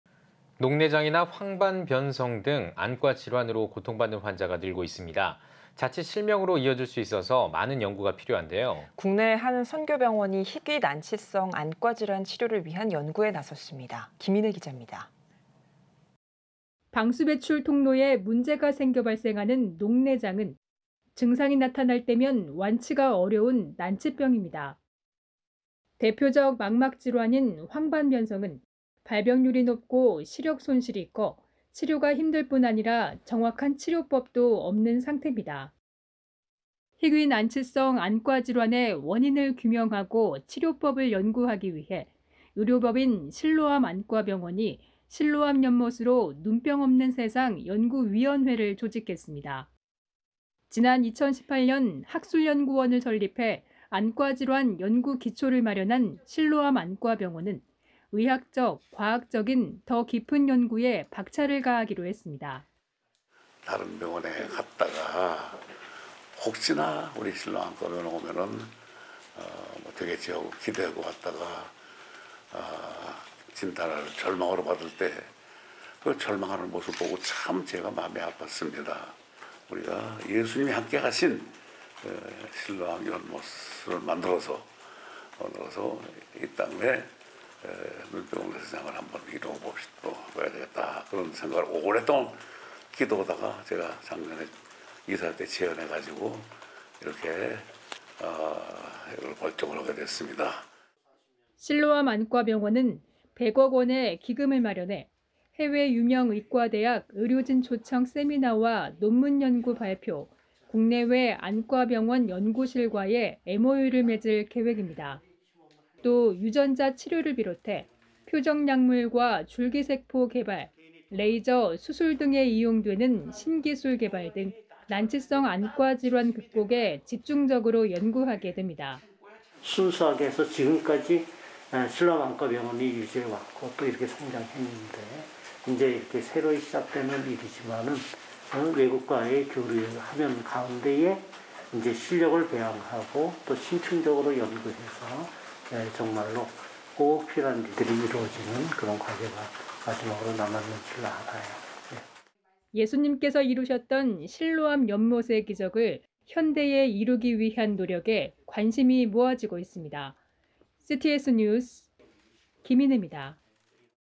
희귀난치성 안질환 연구로 눈병 없는 세상 만듭니다! (김인애) l CTS뉴스
앵커: 녹내장, 황반병성 등 안과질환으로 고통 받는 환자가 늘고 있습니다. 자칫 실명으로 이어질 수 있어서 의학적, 과학적인 깊은 연구가 필요한데요.

앵커: 국내의 한 선교병원이 희귀난치성 안과질환 치료를 위한 연구에 나섰습니다. 김인애 기잡니다.

방수 배출 통로에 문제가 생겨 발생하는 녹내장은 증상이 나타날 때면 완치가 어려운 난치병입니다.
대표적 망막질환인 황반변성은 발병률이 높고 시력 손실이 커 치료가 힘들뿐 아니라 정확한 치료법도 없는 상탭니다.

희귀난치성 안과질환의 원인을 규명하고 치료법을 연구하기 위해 의료법인 실로암안과병원이 ‘실로암 연못으로 눈병 없는 세상 연구위원회’를 조직했습니다.

지난 2018년 학술연구원을 설립해 안과질환 연구 기초를 마련한 실로암안과병원은 의학적, 과학적인 더 깊은 연구에 박차를 가하기로 했습니다.

김선태 원장 / 의료법인 실로암안과병원
다른 병원에 갔다가 혹시나 우리 실로암안과병원에 오면 되겠지 기대하고 왔다가 진단을 절망으로 받을 때 절망하는 모습을 보고 참 제가 마음이 아팠습니다  우리가 예수님이 함께 하신 실로암 연못을 만들어서 이 땅 위에 눈병 없는 세상을 한번 이뤄 봐야 되겠다는 생각을 오랫동안 기도하다가 제가 작년에 이사회 때 제안해서 (연구위원회를) 발족하게 됐습니다

실로암안과병원은 100억원의 기금을 마련해 해외 유명 의과대학 의료진 초청 세미나와 논문연구 발표, 국내외 안과병원 연구실과의 MOU를 맺을 계획입니다. 또, 유전자 치료를 비롯해 표적약물과 줄기세포 개발, 레이저 ․ 수술 등에 이용되는 신기술 개발 등 난치성 안과질환 극복에 집중적으로 연구하게 됩니다.

손신철 위원장 / 실로암 연못으로 눈병 없는 세상 연구위원회
순수하게 해서 지금까지 실로암안과병원이 유지해 왔고 또 이렇게 성장했는데 새로이 시작되는 일이지만 외국과의 교류 하는 가운데 실력을 배양하고 심층적으로 연구해서 정말로 꼭 필요한 일들이 이뤄지는 과제가 마지막으로 남았는줄 알아요

예수님께서 이루셨던 실로암 연못의 기적을 현대에 이루기 위한 노력에 관심이 모아지고 있습니다. CTS뉴스 김인애입니다.